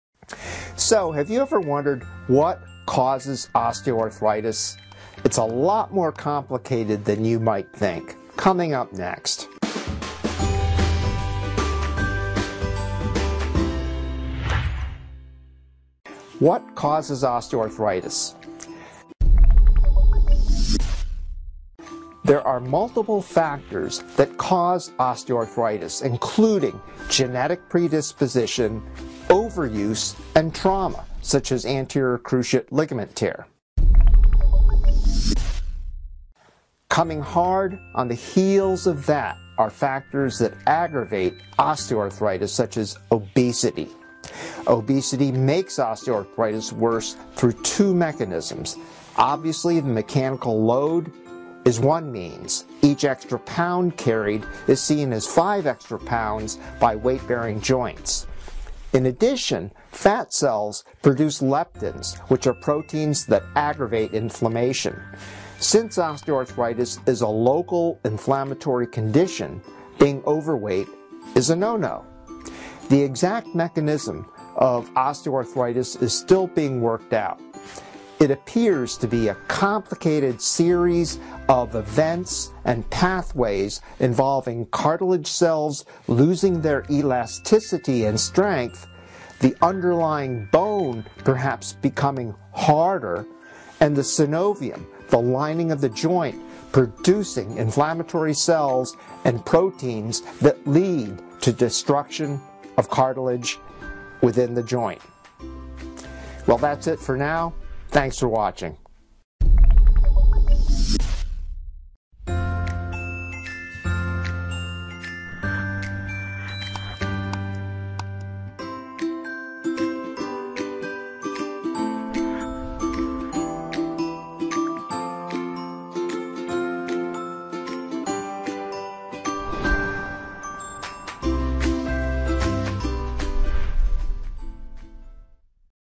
Osteoarthritis | what causes osteoarthritis
So have you ever wondered what cause osteoarthritis.  It’s a lot more complicated than you might think… next
What causes osteoarthritis?
There are multiple factors that cause osteoarthritis including genetic predisposition, overuse, and trauma, such as anterior cruciate ligament tear.  Coming hard on the heels of that are factors that aggravate osteoarthritis such as obesity.  Obesity makes osteoarthritis worse through two ways.  Obviously the mechanical load is one means- each extra pound carried is seen as 5 extra pounds by weight-bearing joints.  Fat cells produce leptins, proteins that aggravate inflammation.  Since osteoarthritis is a local inflammatory condition, being overweight is a no-no.  The exact mechanism of osteoarthritis is still being worked out.  It appears to be a complicated series of pathways involving cartilage cells losing their elasticity, the underlying bone perhaps becoming harder, and the synovium- the lining of the joint- producing inflammatory cells that lead to destruction within the joint.